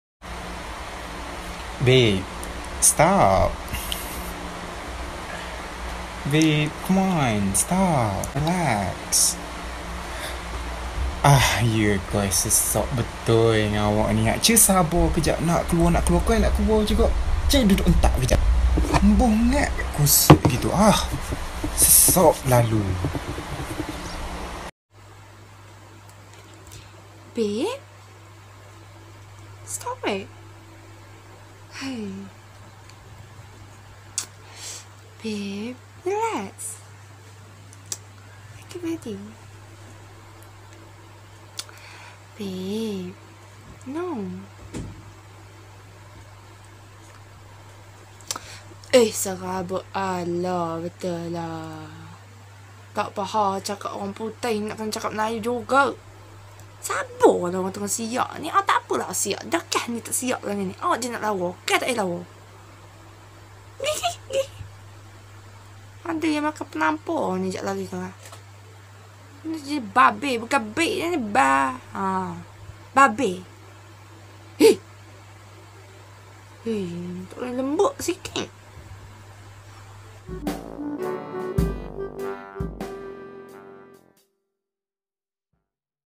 MAROH PASANGAN VERSI TEMERLOH & LEPEH
Bila orang Temerloh dengan orang Lepeh maroh pasangan yang xfaham bahase 😑

p/s: tapi english keh mmg broken la mintak maaf lah yek demeh